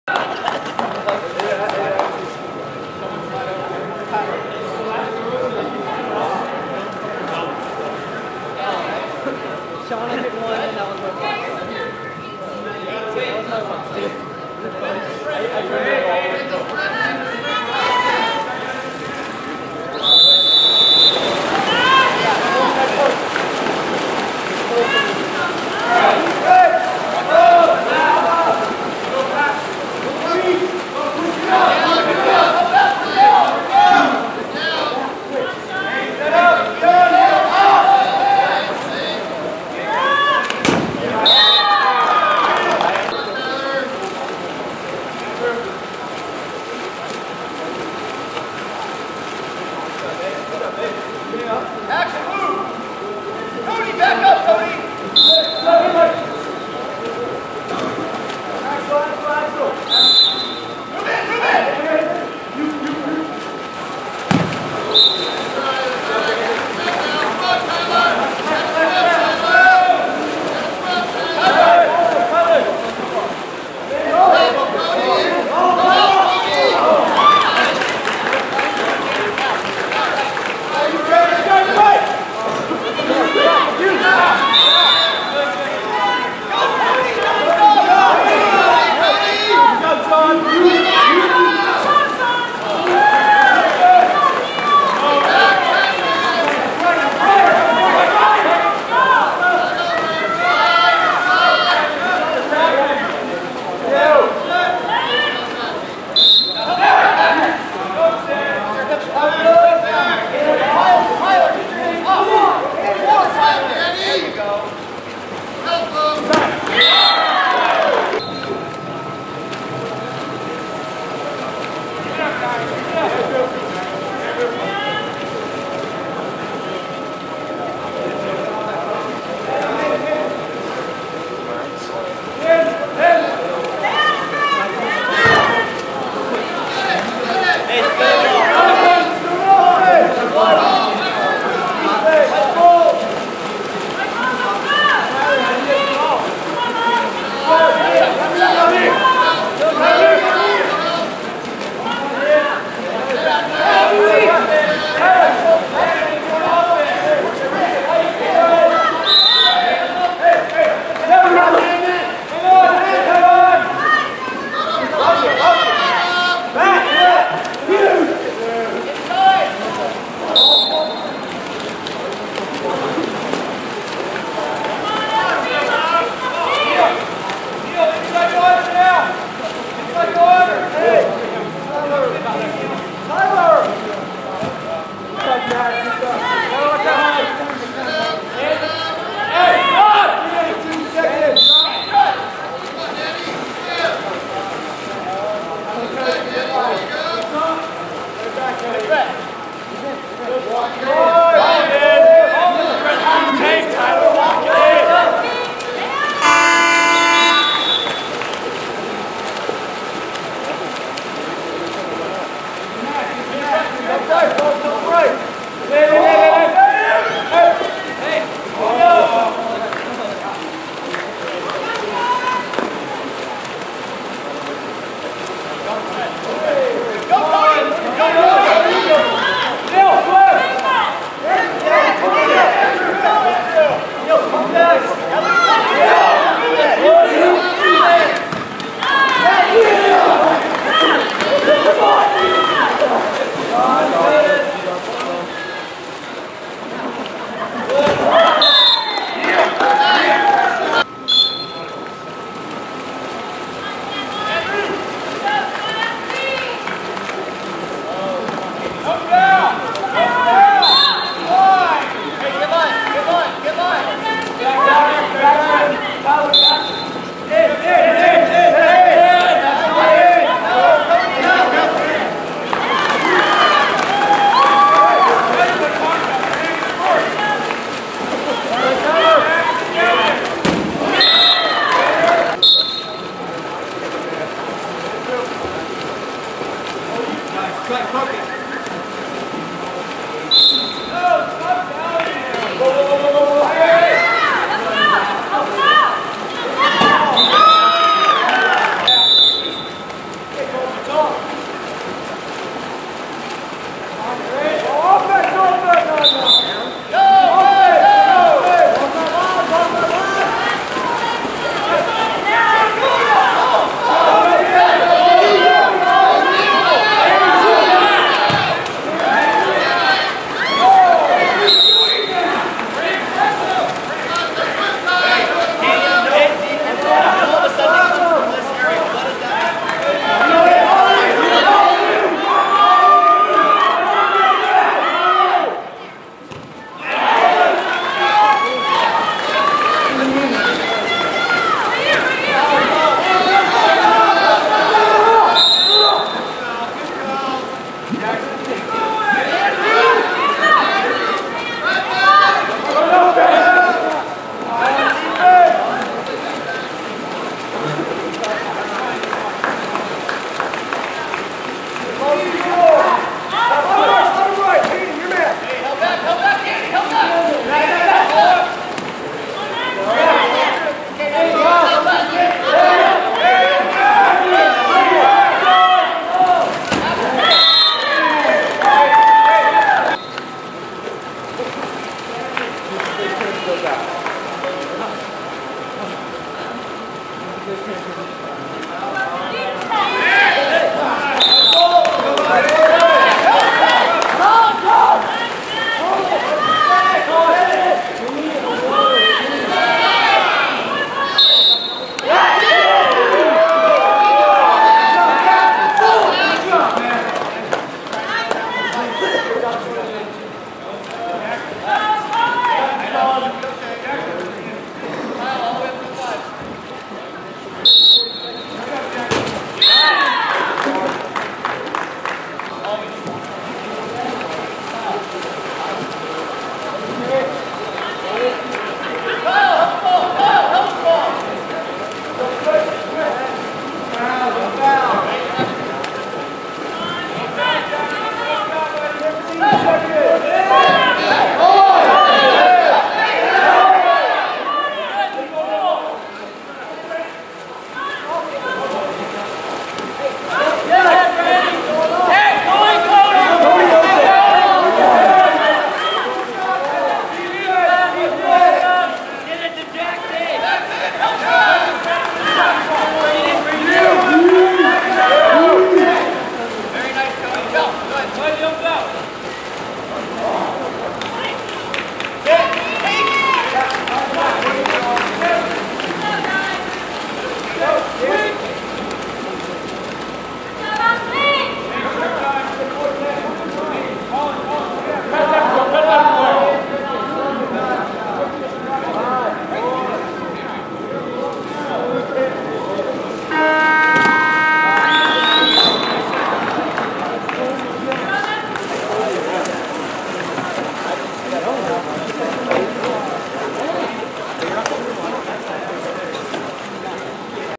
SPHS V vs. San Juan Hills, September 27, 2013 - Q2
Varsity Fall Classic - Game 3 (at Grossmont High School)